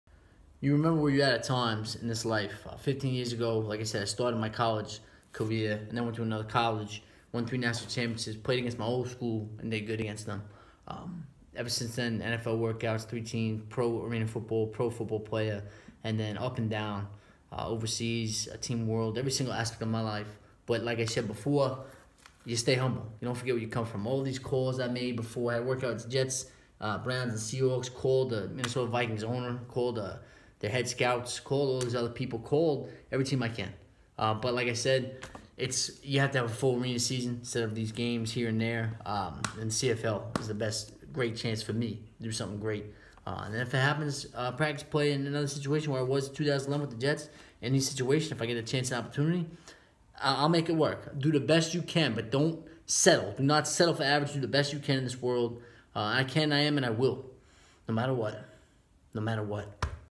August 13⚒🎥💣🏈💚➕⛪️15 Years ago I started my College Football Journey,Now At Pro Level;Ups and do
August 13⚒🎥💣🏈💚➕⛪️15 Years ago I started my College Football Journey,Now At Pro Level;Ups and downs all over the place but with experience and make me better as a person;player it’s all worth it;NYJ;CLE;SEA,I’ve done what I can do far and full arena season and 13 goals are huge for me and my son;Minn,CFL that will come if it does;Control the possible;

Goals In No Order:1.Have my son go to a game I play in and get paid for to watch his father score a TD;2.
Be the best Father I can be to my son even with not as much financial fortune as others and to not make that any excuse..Any guy can have a kid but it takes a MAN to raise one;
3.Be the best guy I can be to Renee..she does a lot not just for me but her son as well.
4.Get in or get a chance for a Marvel movie or luck should have it to start out in ones that I would be great to be apart of(Marvel,John Wick 3;KKFOS 2,ANY type of Friday night lights movies,Pixar movies,Mad max and Quinten Tarantino type movies)
5.Do what I can for money while this is going on..save what I can with football and try to take the ultimate step for a CFL Camp after the Pro NFL workouts,national championships,Pro TD so my Son Can know you believe in yourself and you can do it
6.Train as hard as I can like I have been,Adjust it,Best player I can be,train smart and quality not quantity
7.Be Humble..especially while helping homeless
8.Do not forget where I came from;Elmont NY..HS ball at Winter Springs in FL,2 Colleges,3 Championships at Mount Union
9.Continue not to back down from anyone;make all that hard work be present at practice and in games
10.If ANY free time and I go to baseball games catch a HR on fly for my som(Already caught and got 16 Game HR and 1 Foul HR ,all at Rays games and Indians games)
11.Best I can do for my cats Sabertooth; SNOWY
12.Do not forget passed away pets; current Friends who are family from Elmont NY;
13.EVERY SINGLE ONE OF THESE DONE
Your friends know who you are when you make it...But you KNOW who your friends are when you fall and have to get back up
Be BLESSED AND UNSTOPPABLE
Cherish positive around you and make your life LEGENDARY
GRIND;HUSTLE;Does not stop;Daily!!!; August ⚒🏈💚✝️🎥➕⛪️